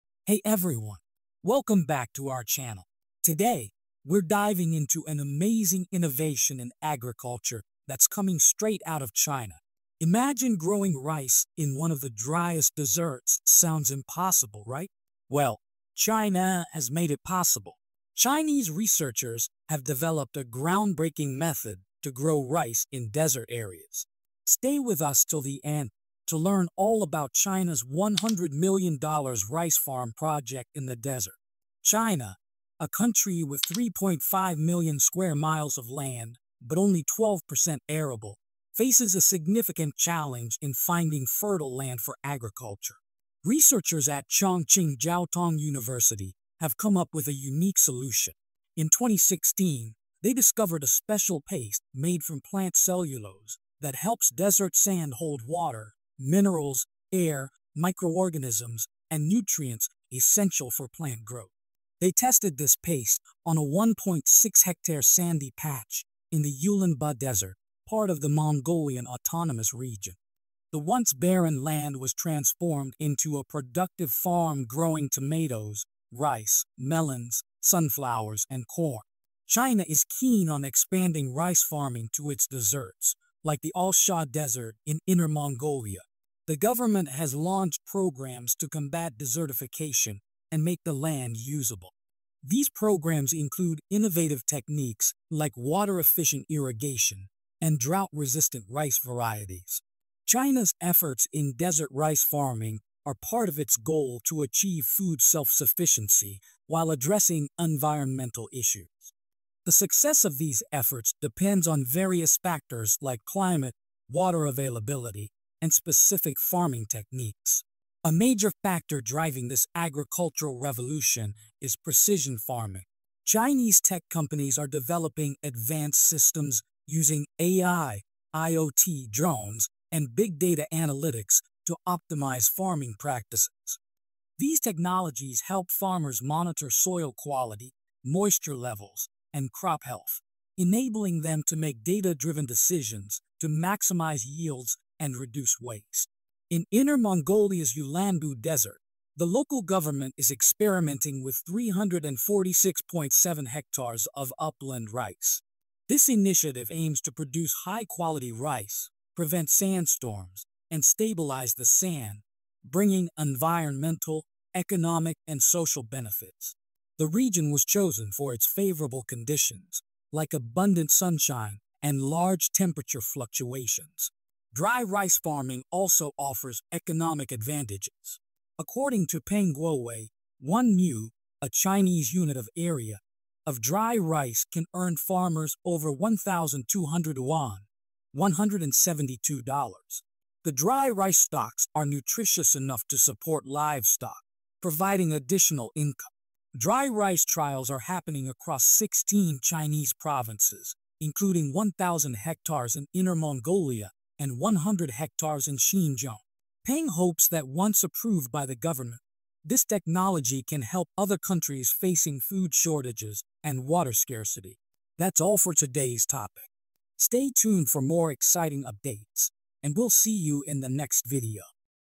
China Grows Rice in the Desert and Amazes Dubai
In a groundbreaking agricultural feat, China has managed to grow rice in the arid deserts, leaving the world and particularly Dubai in awe. This YouTube video delves into the innovative techniques and scientific advancements that have made this seemingly impossible task a reality. Discover how China utilized salt-tolerant rice varieties and advanced irrigation methods to transform barren desert sands into fertile rice paddies.

We'll explore:

The step-by-step process of cultivating rice in such harsh conditions.
The role of cutting-edge technology and genetic engineering.
Interviews with the scientists and agricultural experts behind this project.
The potential global impact of this agricultural breakthrough.
Join us as we uncover how China's ingenuity is not only revolutionizing desert agriculture but also offering new hope for food security in other arid regions worldwide. Watch to see how Dubai, known for its ambitious projects, is now looking towards China's methods with keen interest.